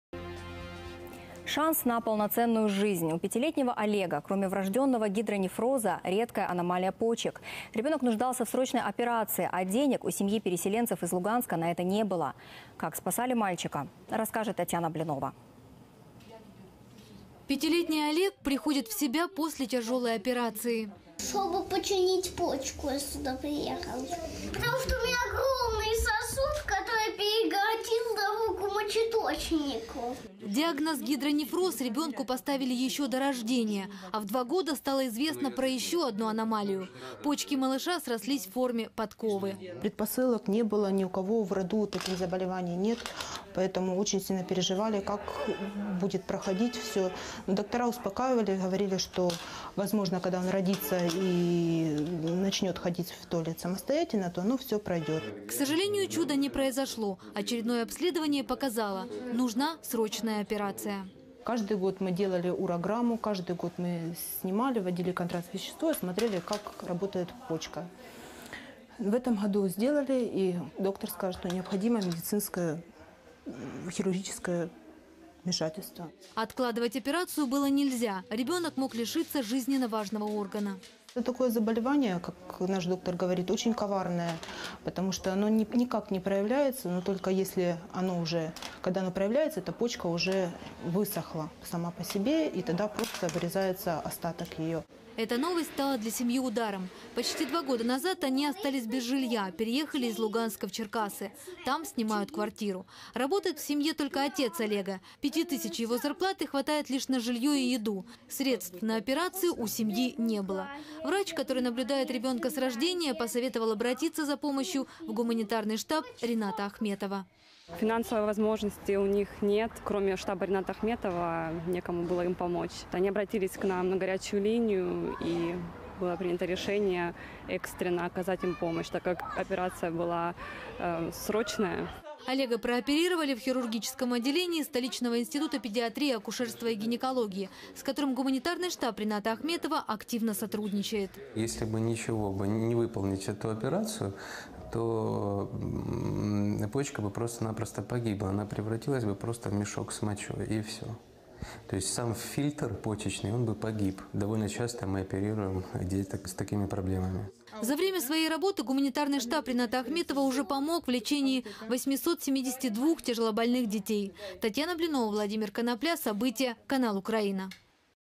Спасти 5-летнего переселенца
У 5-летнего Олега, кроме врожденного гидронефроза, редкая аномалия почек. Ребенок нуждался в срочной операции, а денег у семьи-переселенцев из Луганска на это не было. Как спасали мальчика?